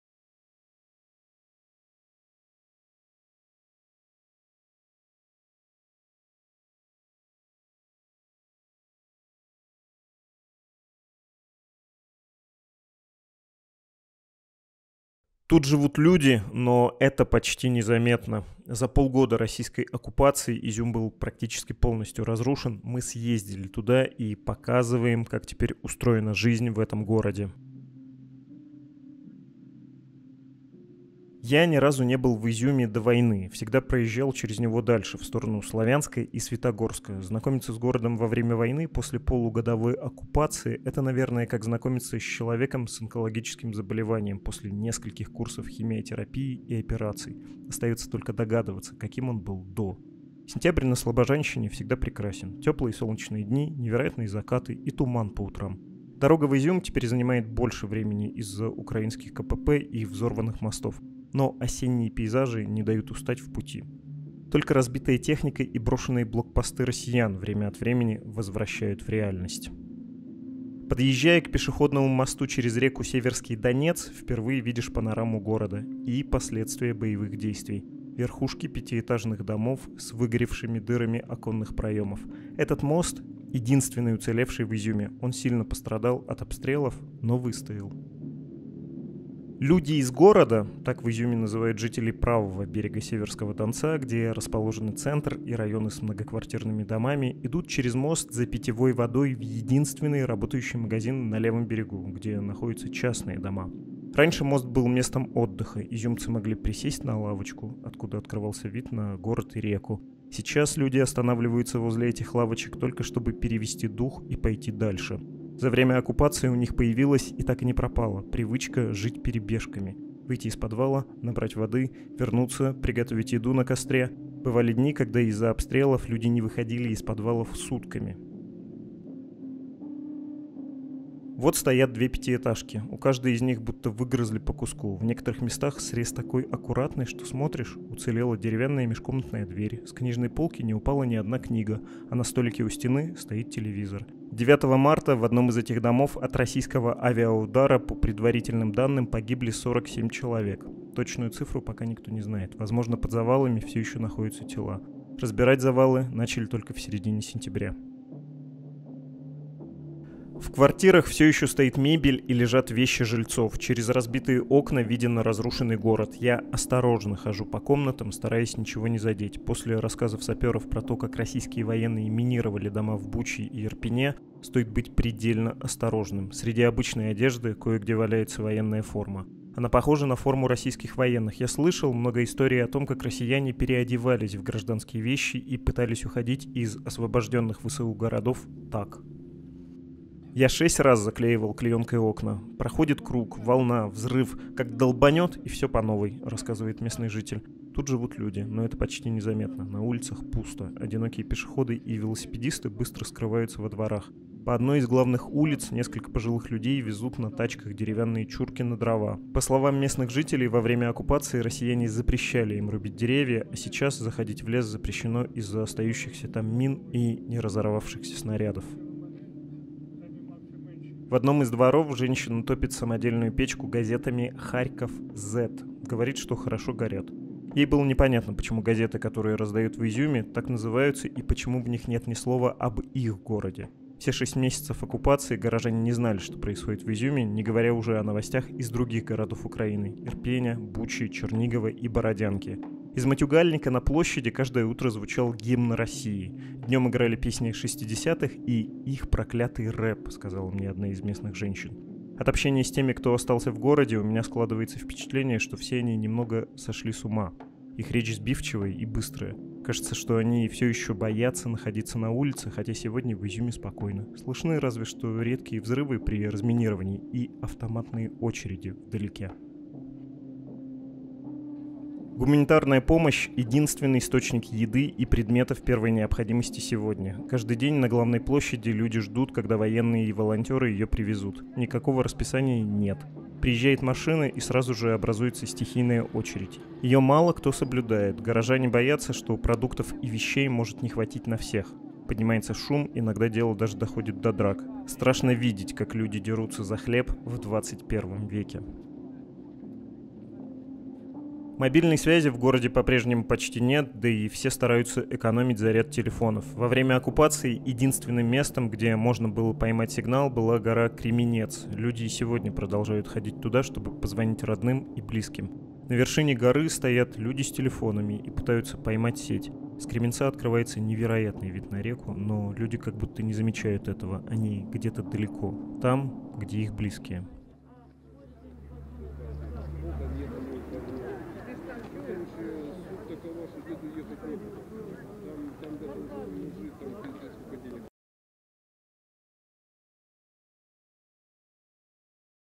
Как выглядит Изюм после полугода российской оккупации
Жители Изюма — небольшого города в Харьковской области — провели под российской оккупацией полгода. За это время город был фактически разрушен. «Медуза» попросила харьковского фотографа Павла Дорогого съездить в Изюм, чтобы показать и рассказать, как теперь устроена жизнь в этом городе. Вот что он увидел.

Это видео было сделано специально для аккаунта «Медузы» в инсте.